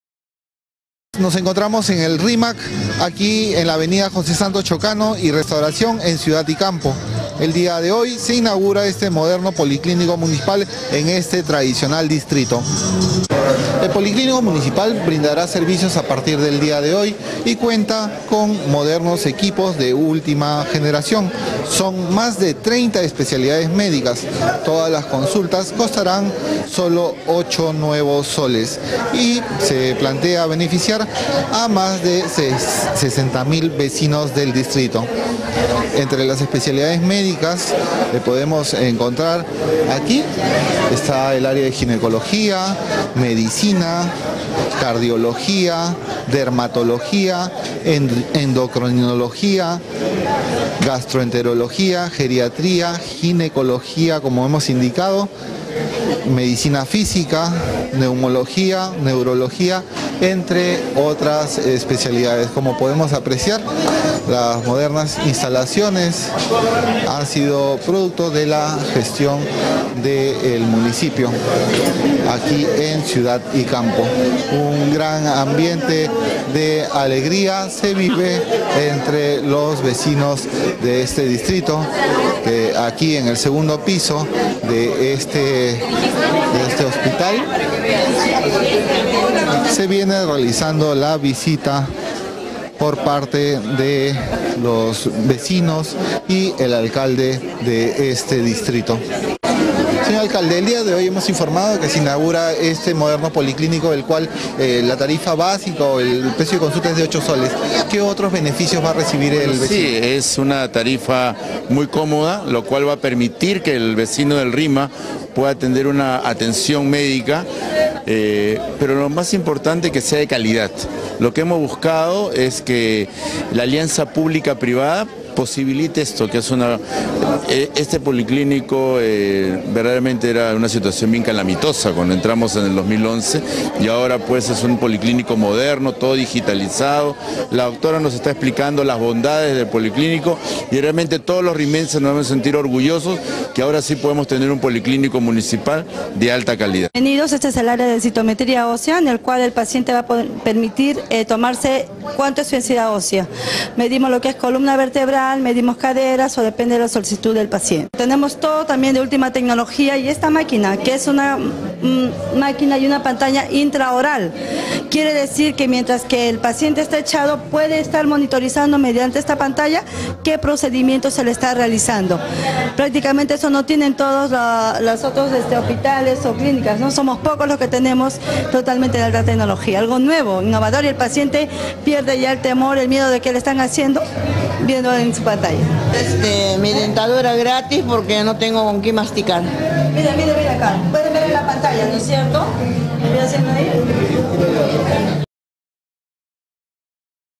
El Rímac tiene nuevo Policlínico
LIMA.- La municipalidad del Rímac inauguró esta mañana un moderno Policlínico en la avenida José Santos Chocano. Las personas que acudan podrán encontrar más de 30 especialidades y las consultas tendrán un precio de 8 soles. Nuestro videoreportero Silvio Buendía estuvo en el lugar y conversó con el alcalde. Veamos.